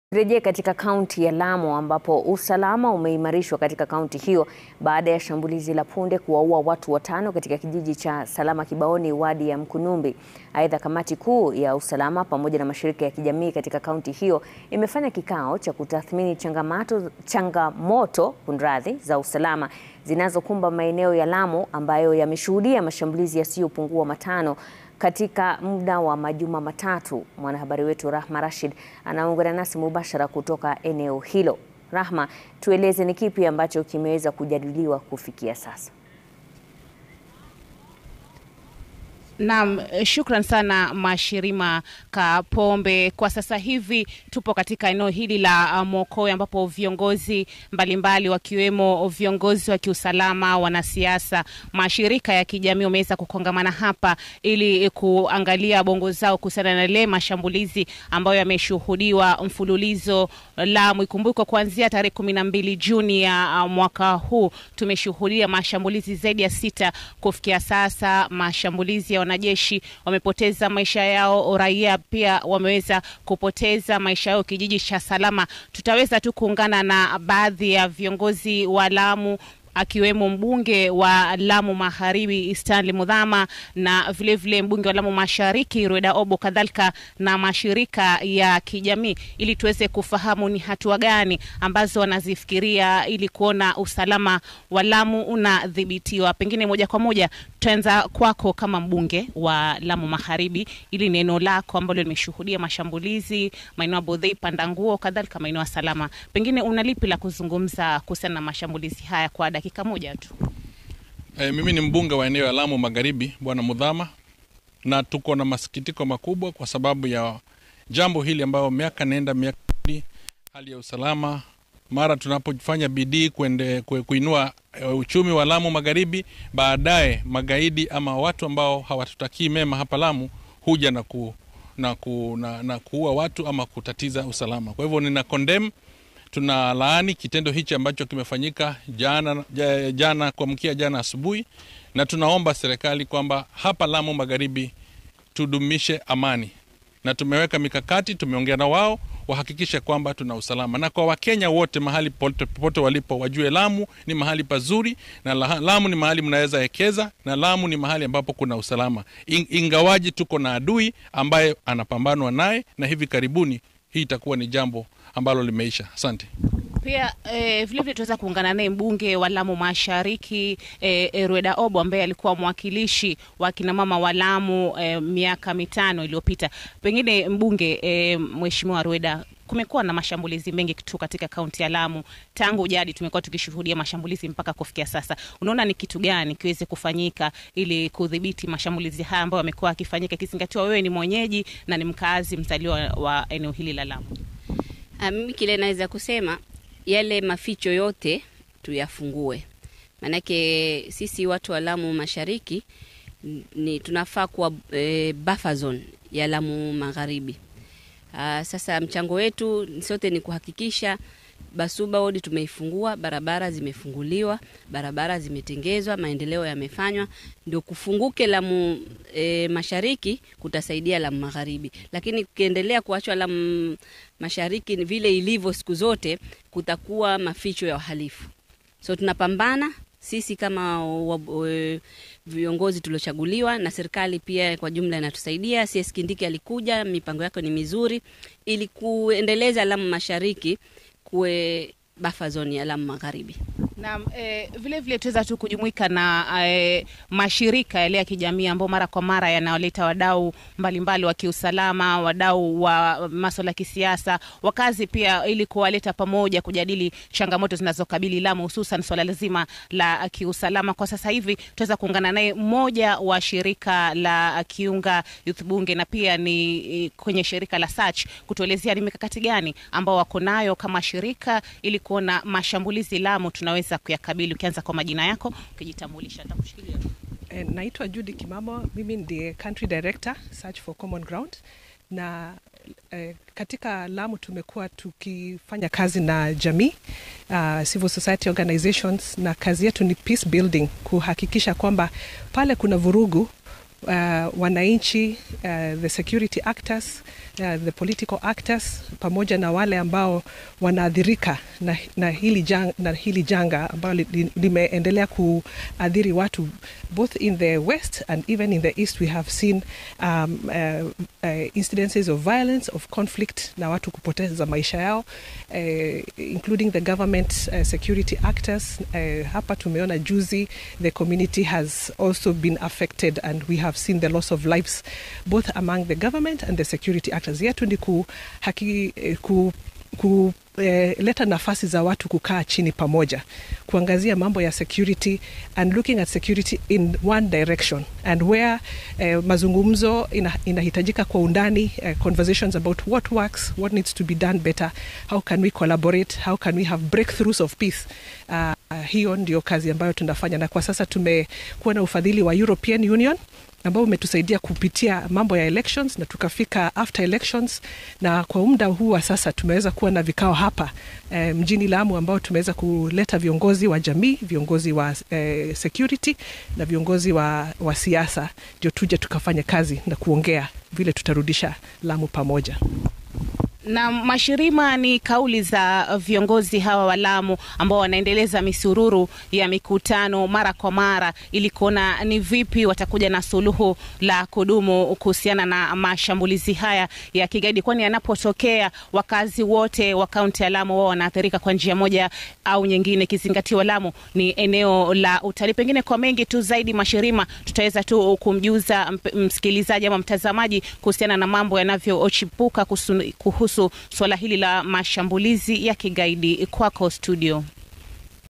Usalama waimarishwa baada ya shambulizi la punde katika kaunti ya Lamu
Usalama umeimarishwa kaunti ya Lamu, baada ya shambulizi la punde kuwaua watu watano katika kijiji cha Salama-Kibaoni wadi ya Mkunumbi. Aidha, kamati kuu ya kiusalama pamoja na mashirika ya kijamii kaunti hiyo imefanya kikao cha kutathmini changamoto za usalama zinazokumba maeneo ya Lamu, ambayo yameshuhudiwa mashambulizi yasiyopungua matano katoka muda wa majuma matatu.